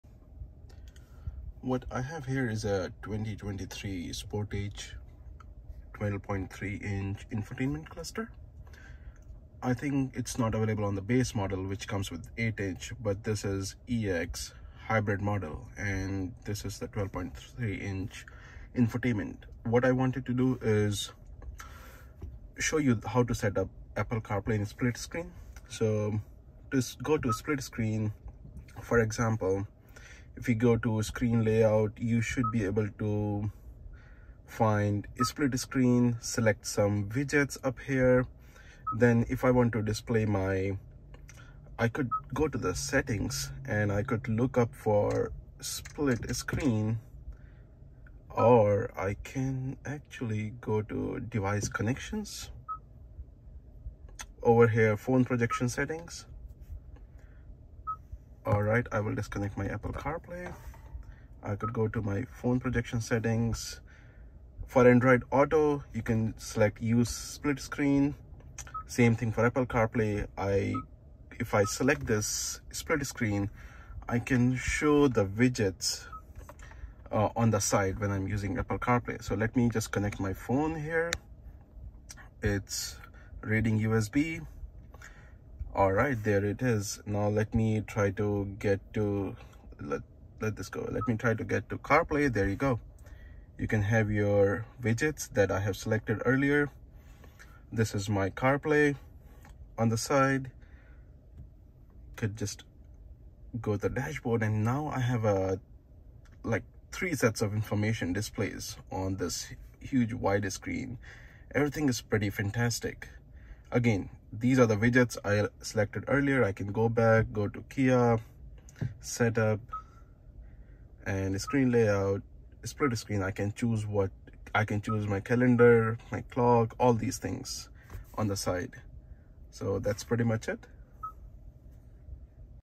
2023 Kia Sportage - Carplay or Android Auto split screen
2023 Kia Sportage has a 12 inch infotainment, I posted this instruction how to use the real estate effectively with Carplay. Basically, using carplay in a split screen mode.